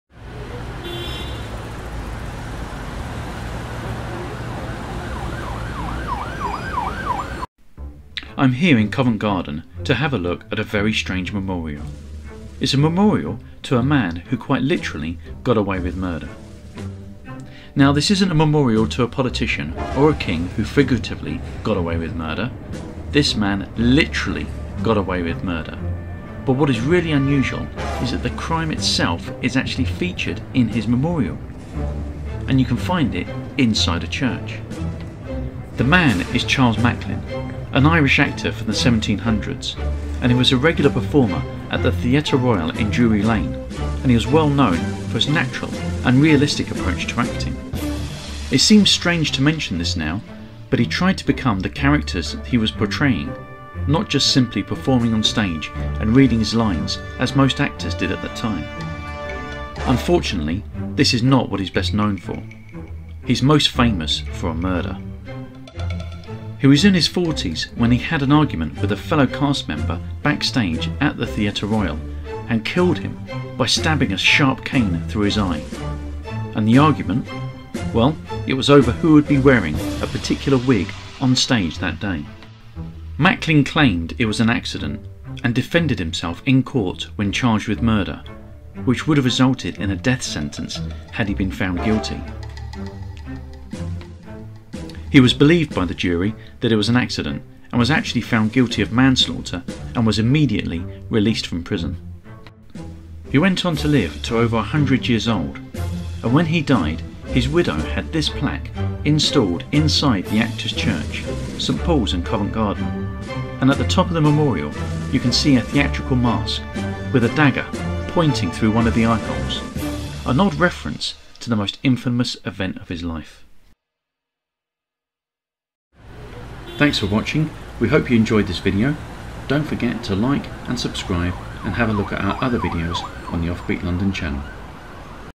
A TRUE CRIME Story: The Memorial To A Man Who Got Away With MURDER
In Covent Garden, London, there is a church, and in that church there is a memorial to a man who got away with murder! What is strange about this memorial is that the murder itself is directly referenced in the memorial.

This is an historical true crime tale!